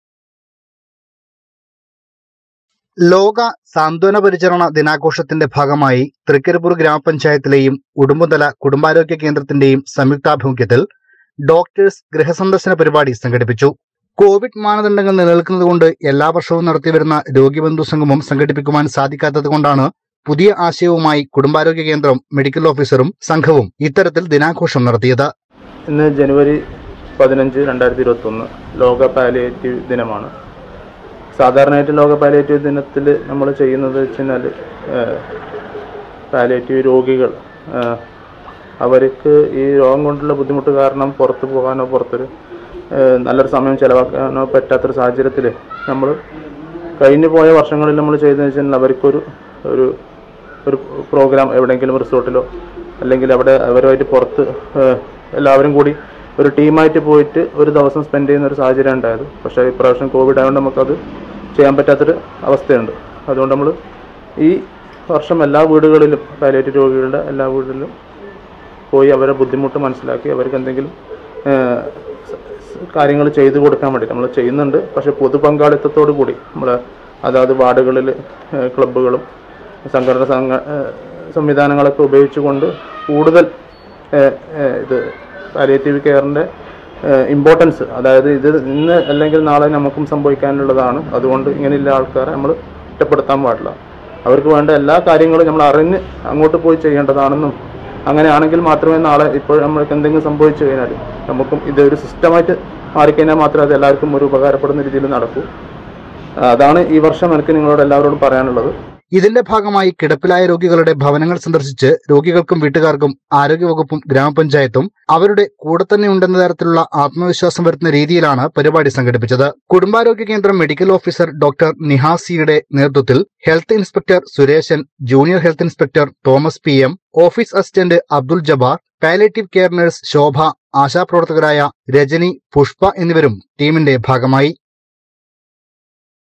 ലോക സാന്ത്വന പരിചരണ ദിനാഘോഷത്തിന് ഭാഗമായി തൃക്കരിപ്പൂർ ഗ്രാമ പഞ്ചായത്തിലെയും ഉടുമ്പുന്തല കുടുംബാരോഗ്
ലോക സാന്ത്വന പരിചരണ ദിനാഘോഷത്തിന് ഭാഗമായി തൃക്കരിപ്പൂർ ഗ്രാമ പഞ്ചായത്തിലെയും ഉടുമ്പുന്തല കുടുംബാരോഗ്യ കേന്ദ്രത്തിൻ്റെയും സംയുക്ത ആഭിമുഖ്യത്തിൽ.
ഡോക്ടേഴ്സ് ഗൃഹസന്ദർശന പരിപാടി സംഘടിപ്പിച്ചു..